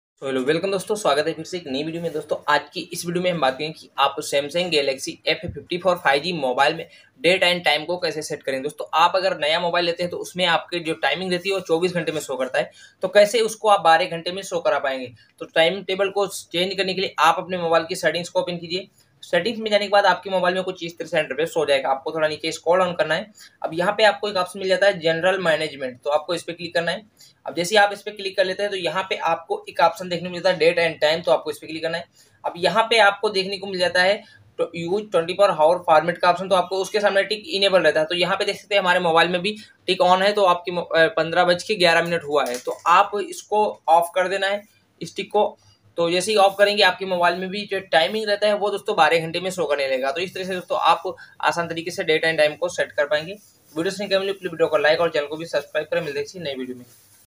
How To Set Time ⏲️ Teble in Samsung f54 5g/Samsung F54 5g me Change date and time setting
Hello friends aaj ki is Video me Maine aap logo ko bataya hai ki Samsung galaxy f54 5g mobile me time table change kaise kare